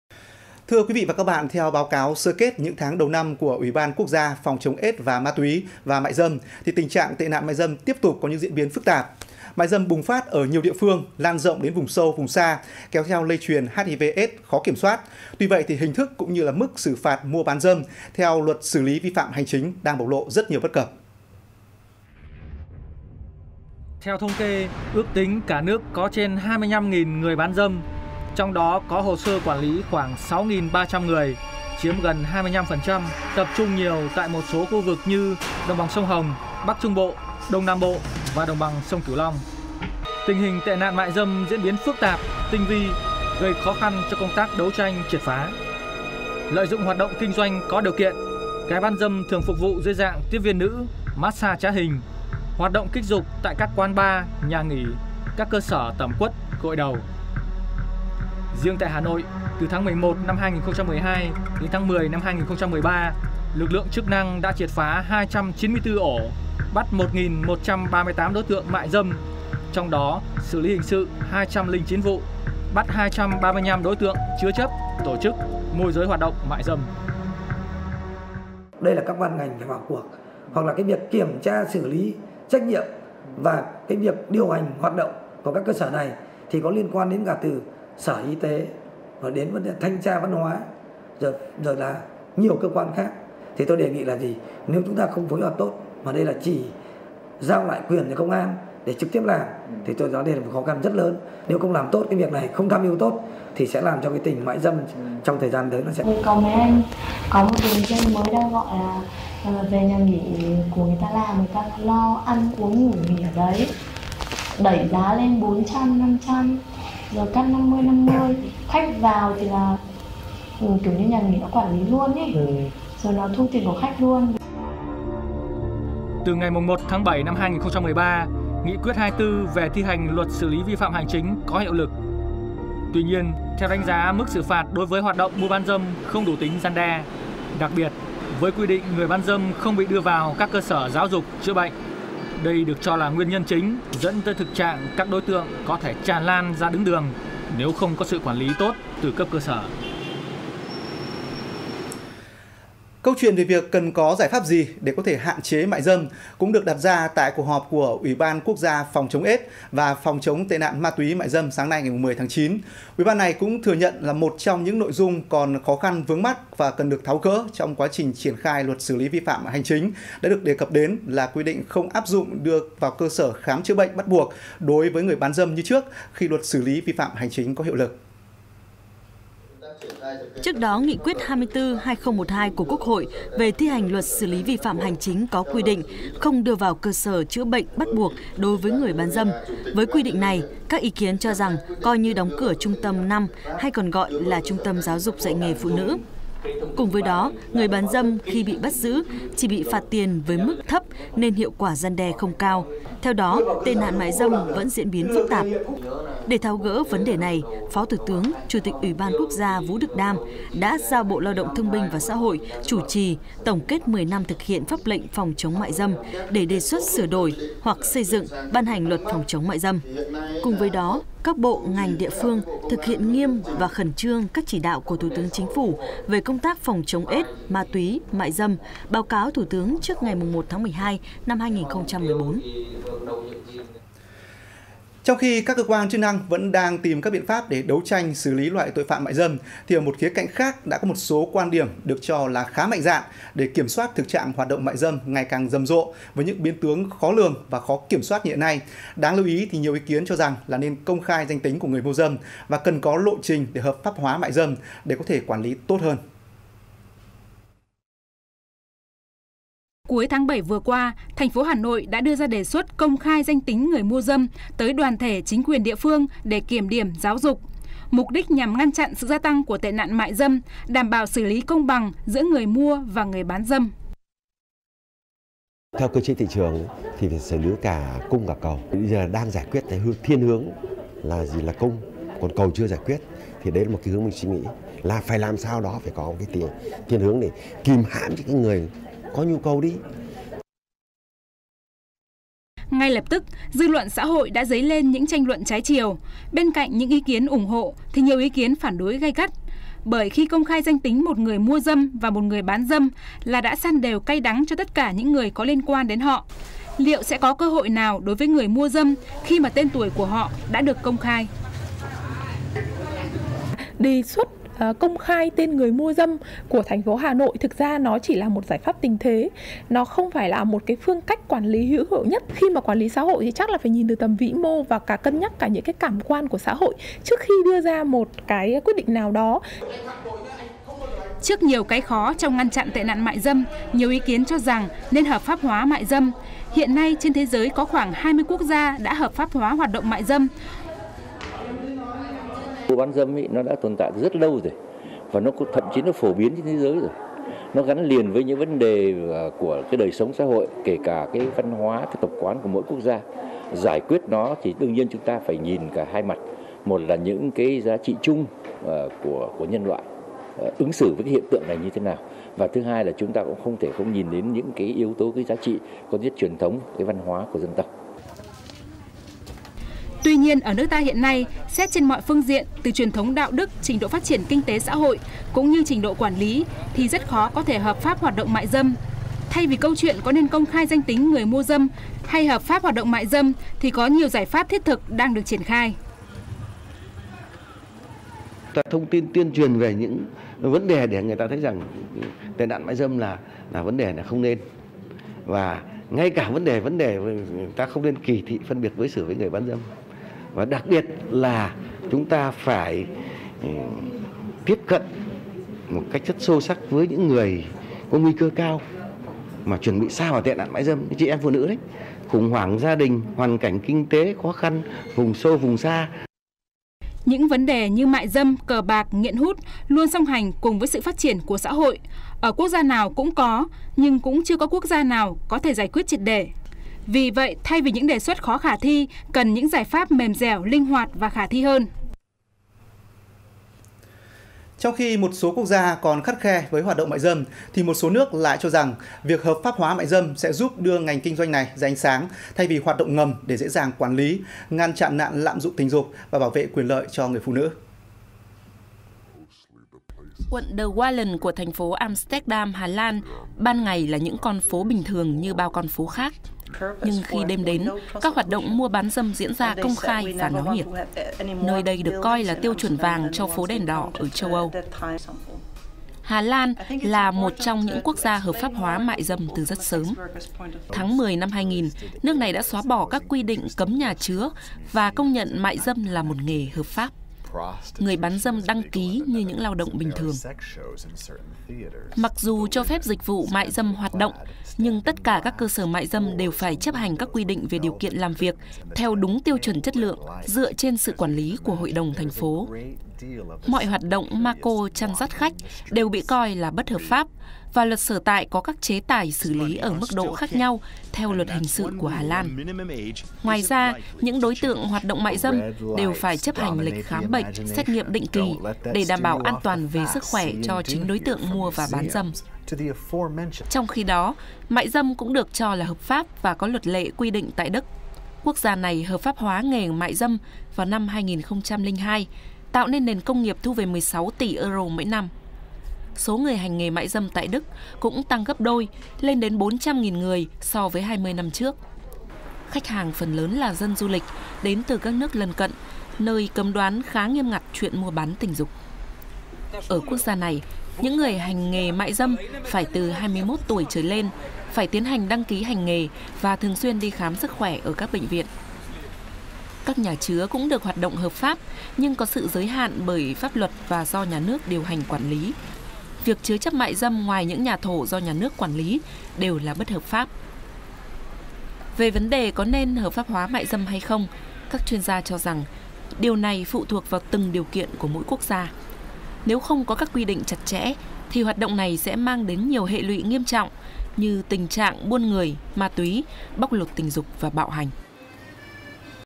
Mại dâm hợp pháp ở Việt Nam: Cần có lộ trình | VTC1
VTC1 | Hoạt động mại dâm có nên được “hợp pháp hoá” tại Việt Nam? Nếu có thì cần có lộ trình như thế nào?